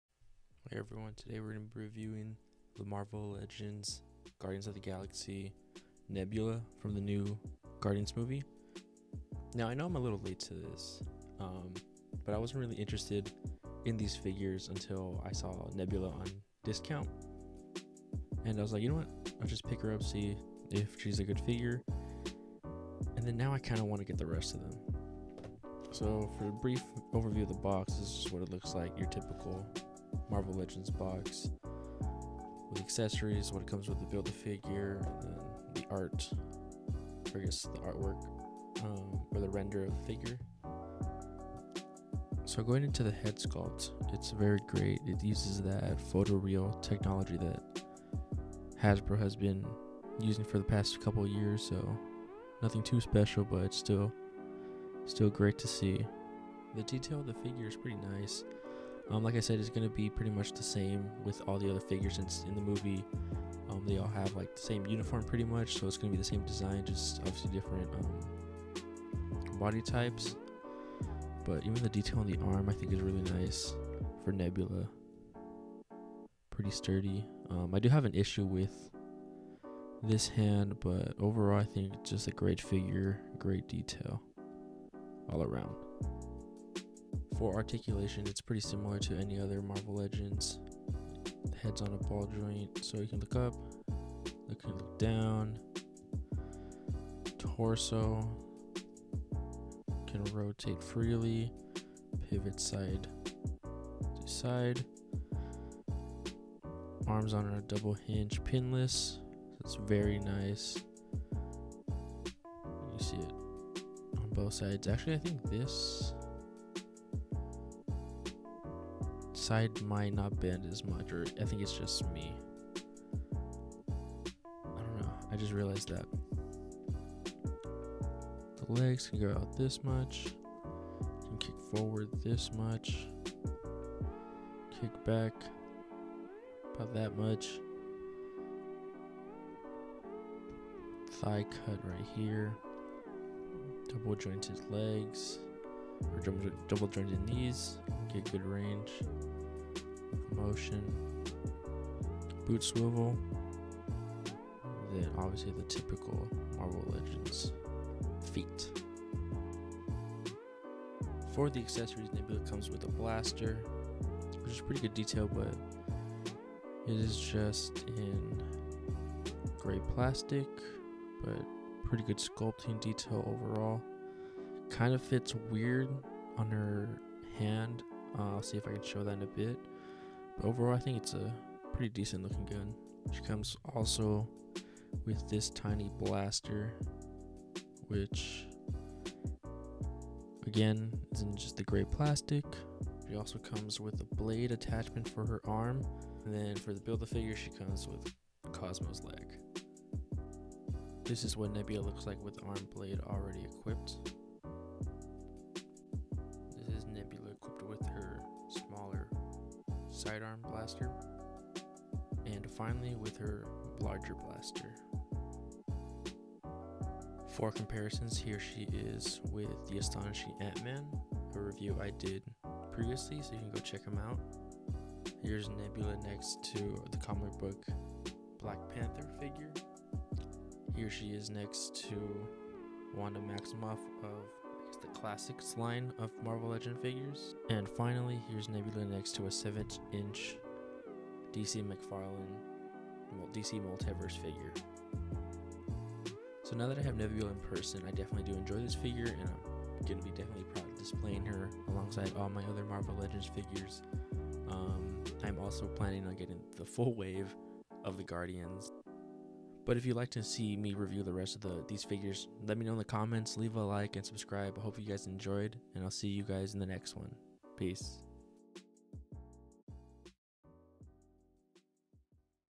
Marvel Legends: Guardians of The Galaxy Vol.3 Nebula
Today I review the Marvel legends Guardians of the galaxy nebula figure.